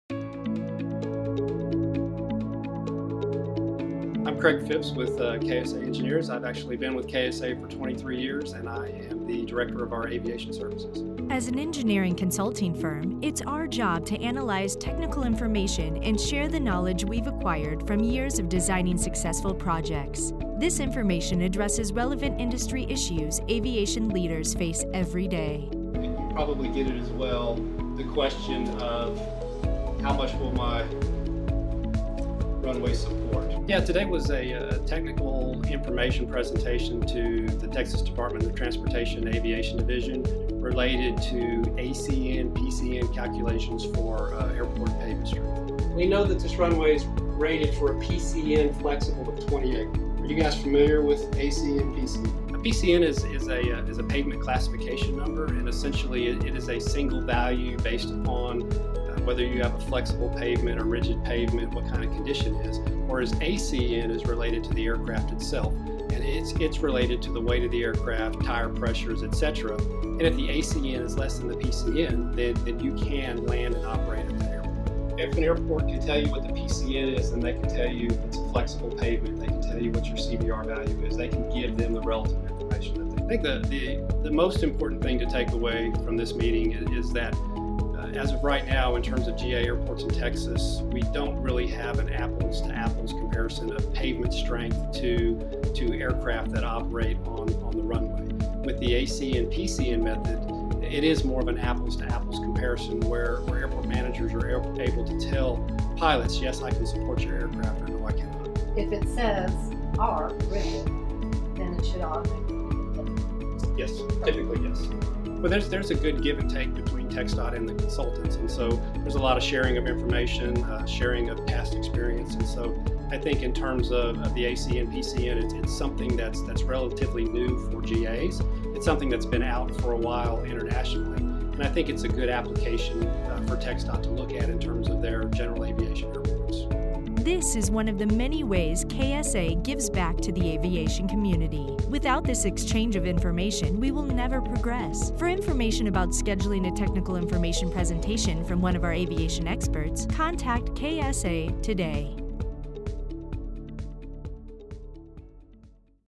Q&A with KSA - Craig Phipps, P.E. Aviation Expert
As experts in aviation related engineering consulting services, it's our responsibility to share our knowledge both with our clients and governing agencies as seen here with TxDOT Aviation staff in Austin, Texas. KSA has over 30 years of experience building airports and airport related infrastructure. It's important to us that the industry benefit from our expertise.